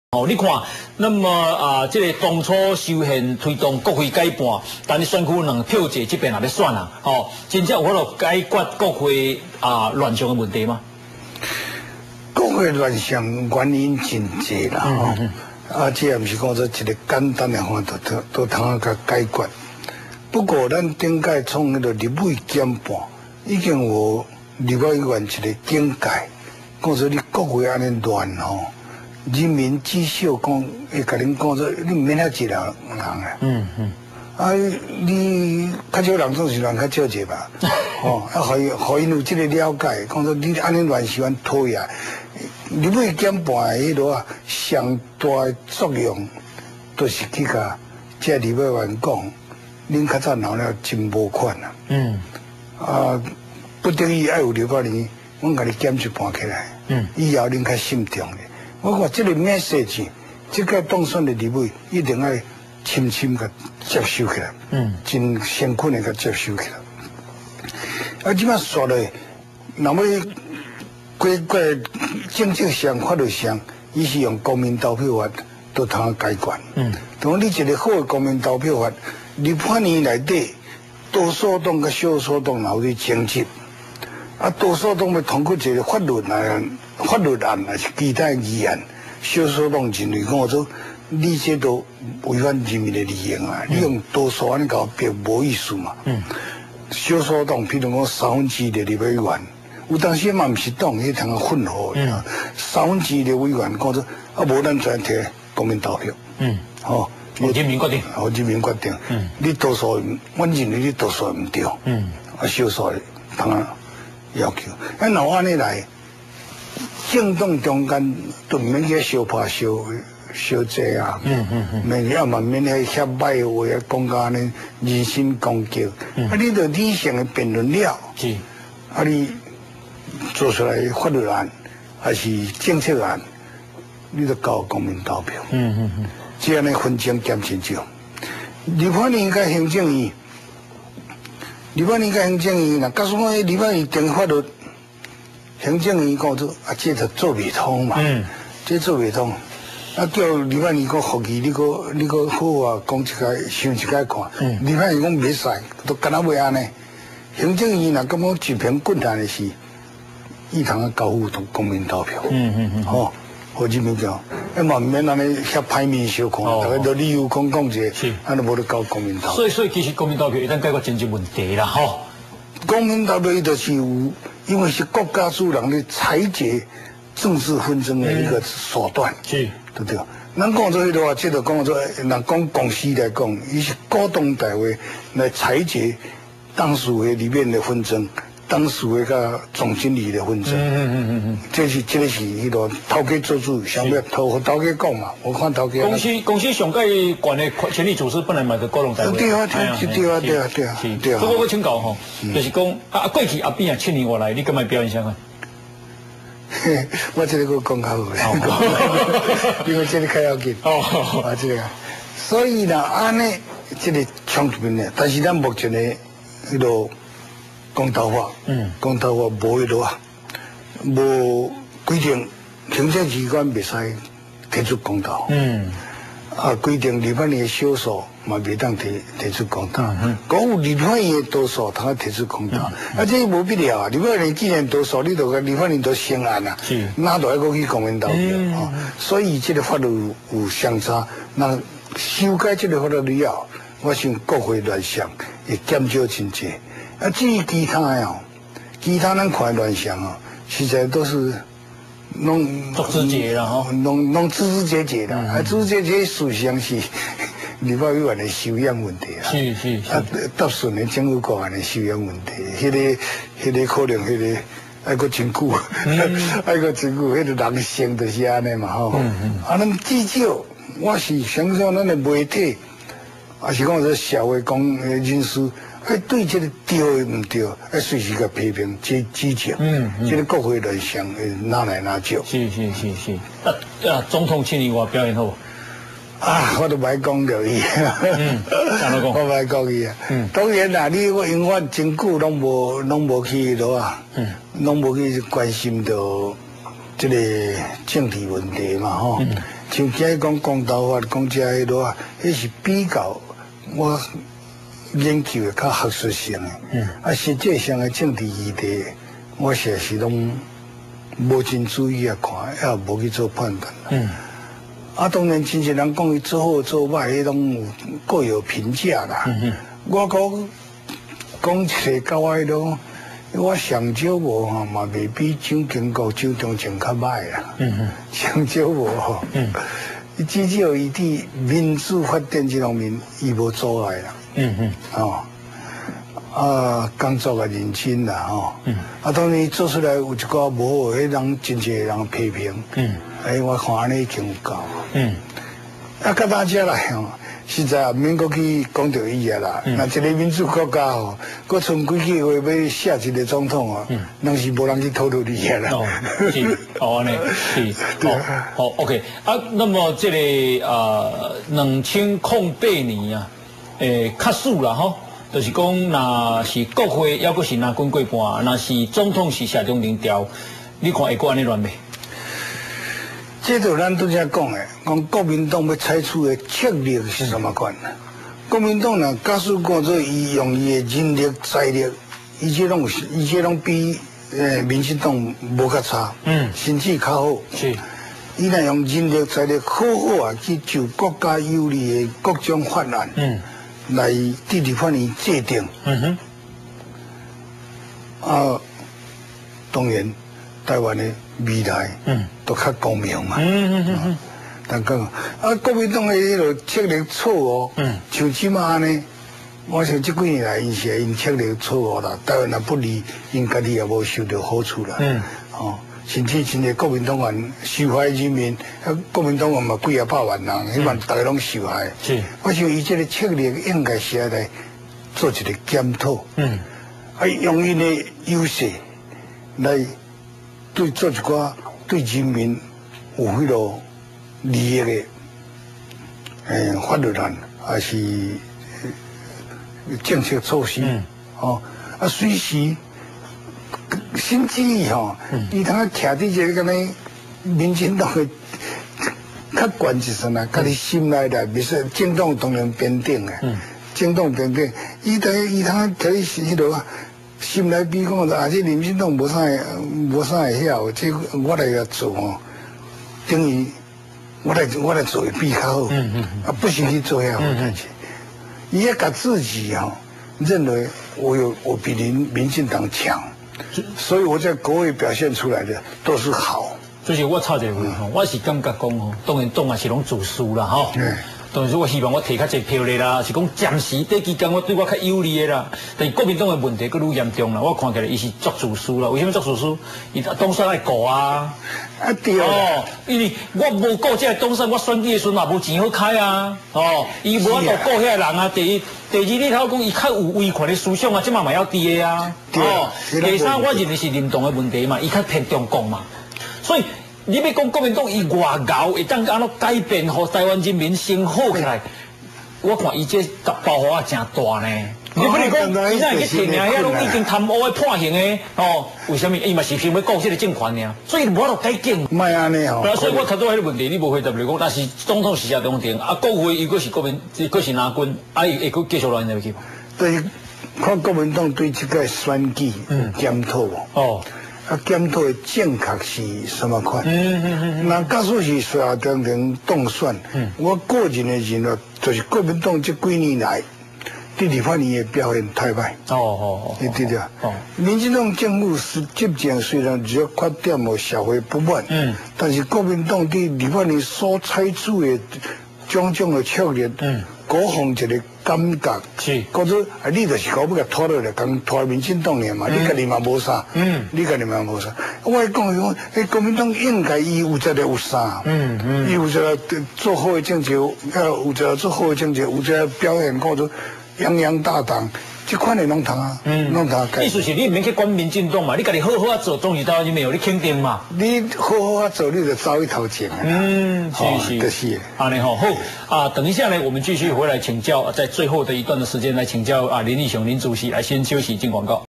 林義雄專訪 (9) 談立委減半,單一選區兩票制,對目前國會亂像的影響與意義.
林義雄專訪(9) 為台灣民主奉獻犧牲的人格者. 談立委減半,單一選區兩票制,對目前國會亂像的影響與意義. 補正公民投票法,落實直接民權,亦可以透過理性辯論後制衡未來國會政治的紛爭. 仍期待國民黨應發揮手上優勢資源,採取正面策略,為台灣做些好事,勿惡意杯葛.